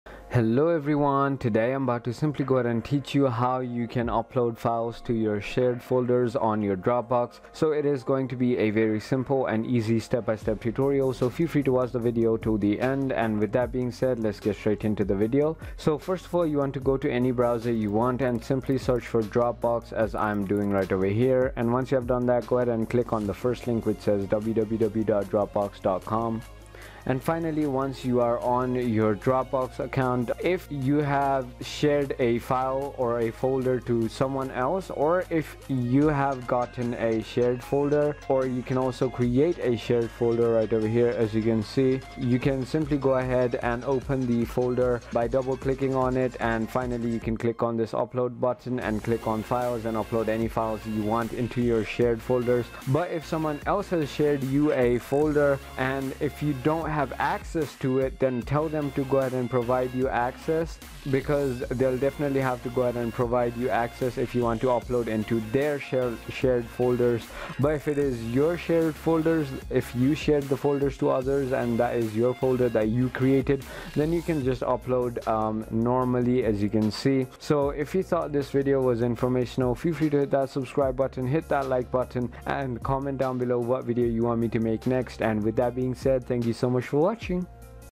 How to Upload Files to Dropbox Shared Folders | 2021
➜ In this video tutorial i will guide you on how you can upload files to shared folders on Dropbox.